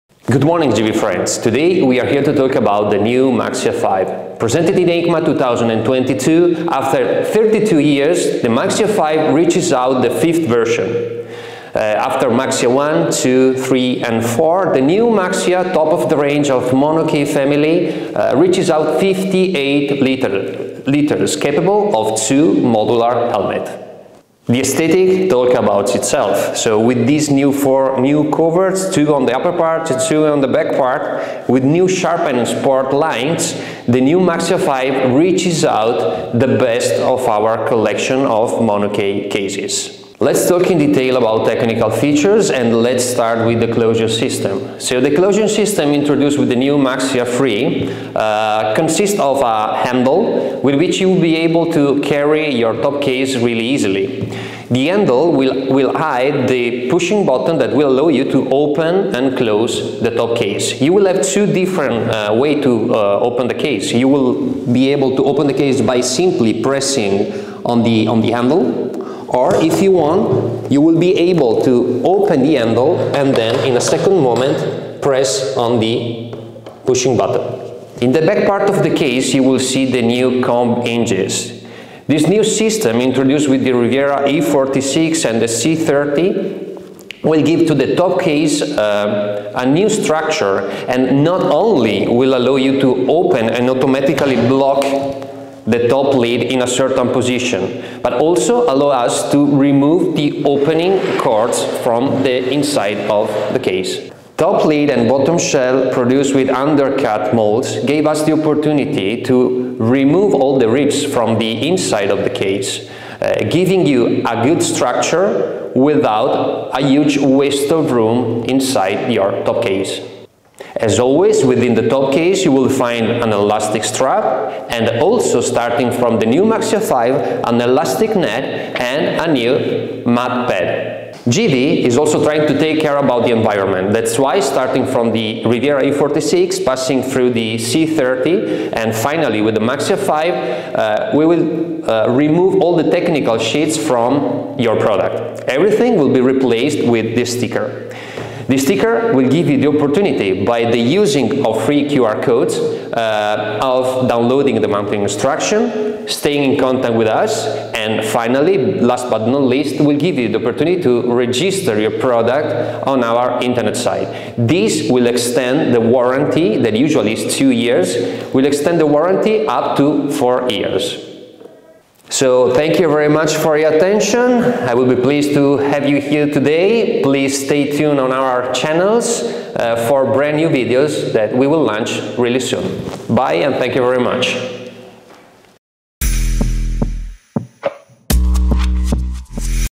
Maleta Maxia 5 GIVI | Vídeo Técnico (audio ingles)
La gama Maxia comenzó en 1991 y ahora este nuevo modelo sitúa la Maxia 5 entre las mejores maletas para moto. La marca GIVI tras años de evolución ha logrado diseñar un top case elegante, con toques futuristas y con la más alta tecnología, que responde a las más altas necesidades.

REDES